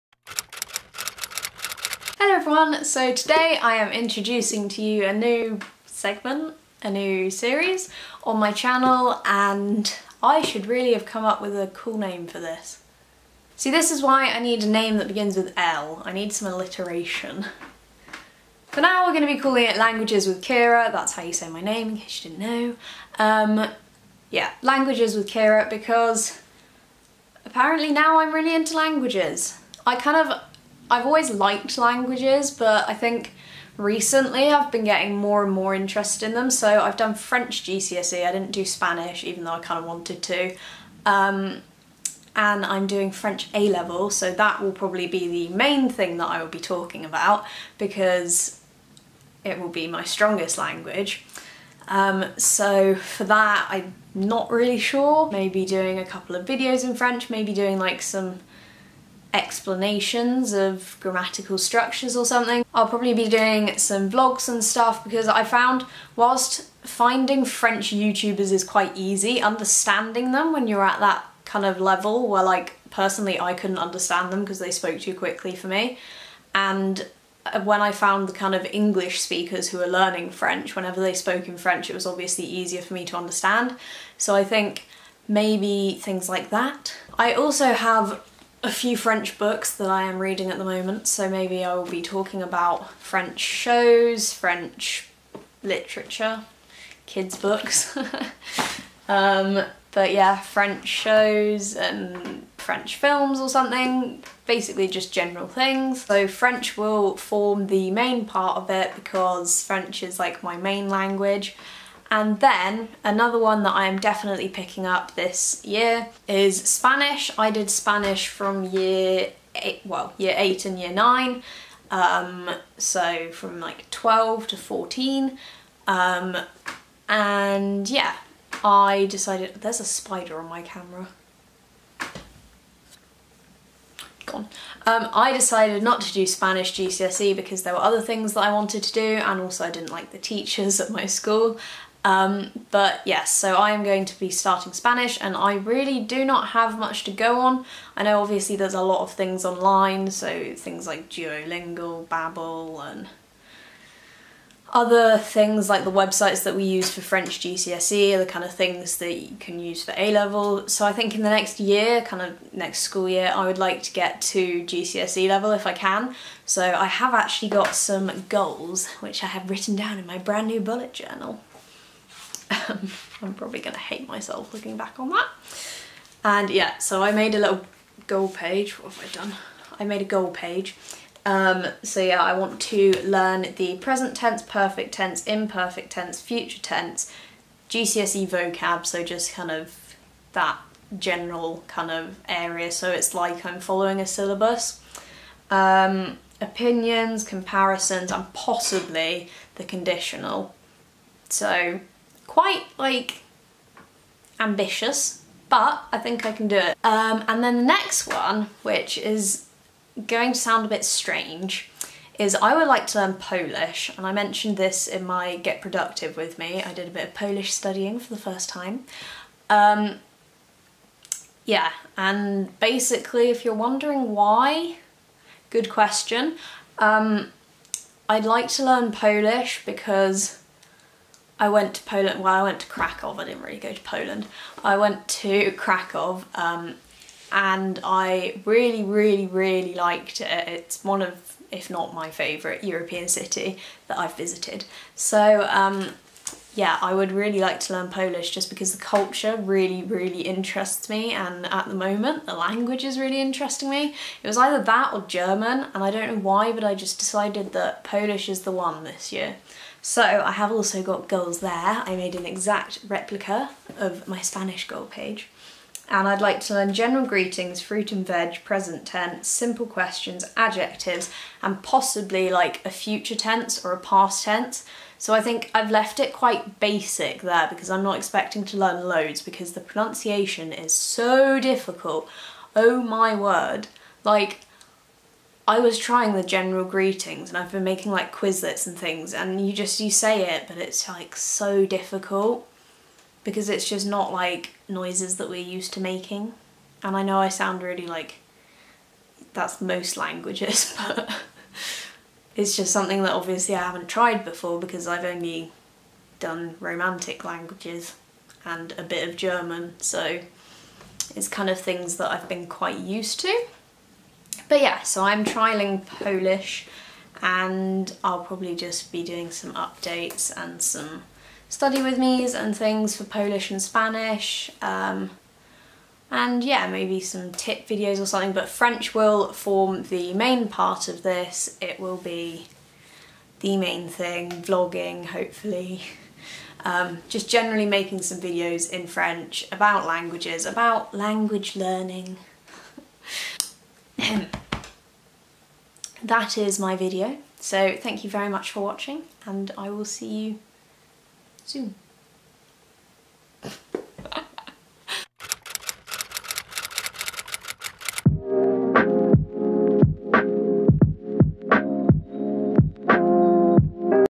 New Series On My Channel? || Languages With Ciara #1
Hope you enjoyed this video explaining my plans for the next few months (and hopefully after that too :D )

Leave any suggestions, requests etc. in the comments

and I'll see ya soon